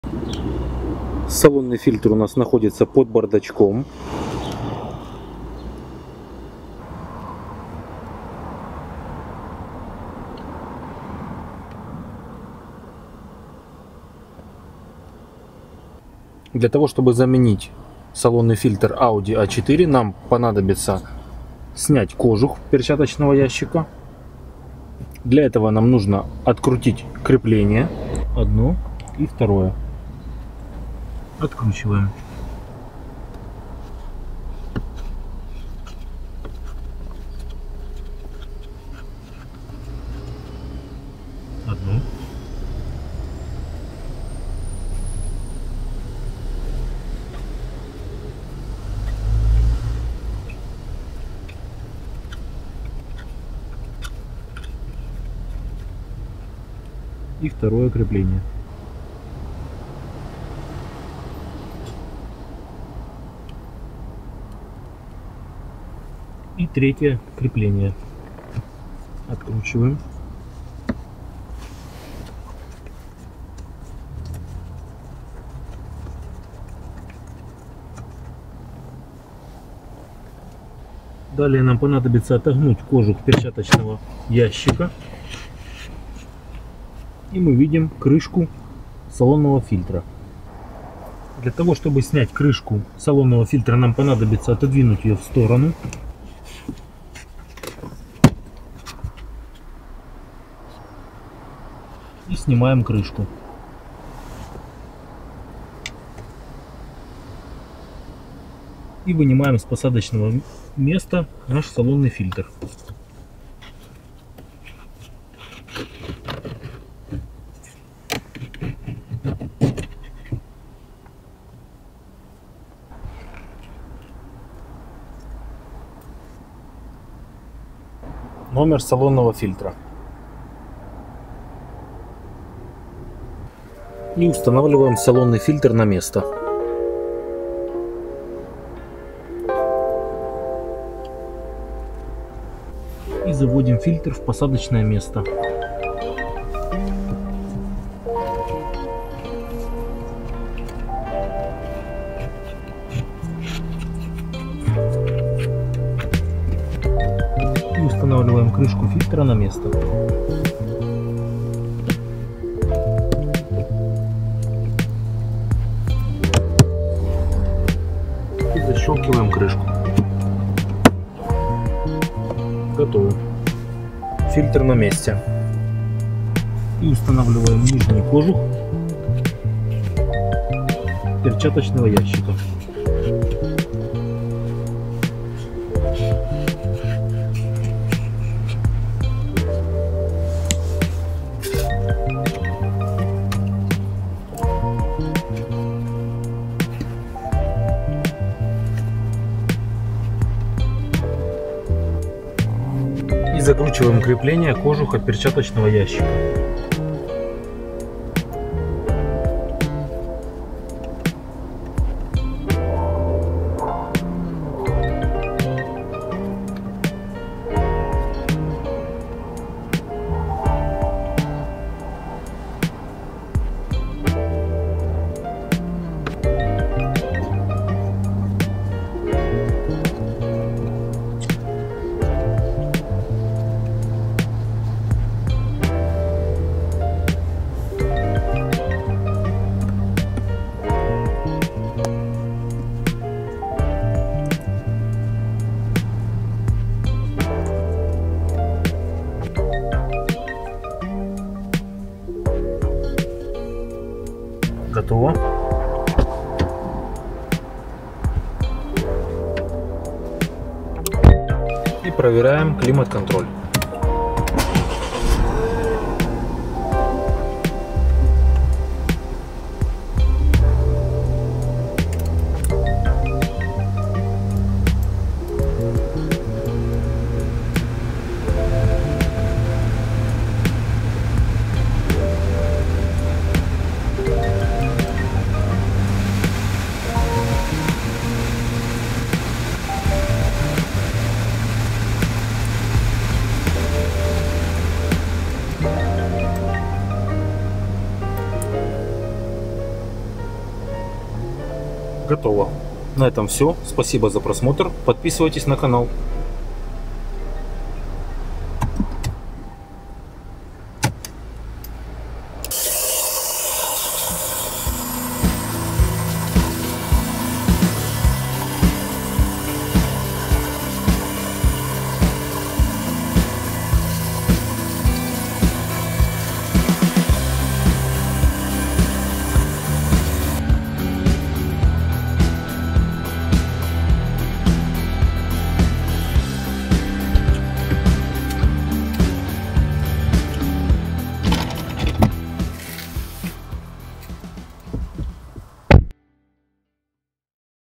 Замена Салонного Фильтра AUDI A4 (B8) Cabin Filter Replacement DIY
Как поменять салонный фильтр Audi A4 B8 замена салонного фильтра самостоятельно. Как заменить салонный фильтр Audi A4 B8 кузов.
Видео урок о замене салонного фильтра подходит для следующих моделей Ауди:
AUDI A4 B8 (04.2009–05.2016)
AUDI A4 B8 Allroad (8KH) (04.2009–05.2016)
AUDI Q5 (8RB) (11.2008–)
AUDI A6 C7 Saloon (4G2, 4GC) (11.2010–)
AUDI A7 Sportback (4GA, 4GF) (10.2010–)
AUDI A5 B8 Coupe (8T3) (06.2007–01.2017)
Как правильно заменить салонный фильтр в Ауди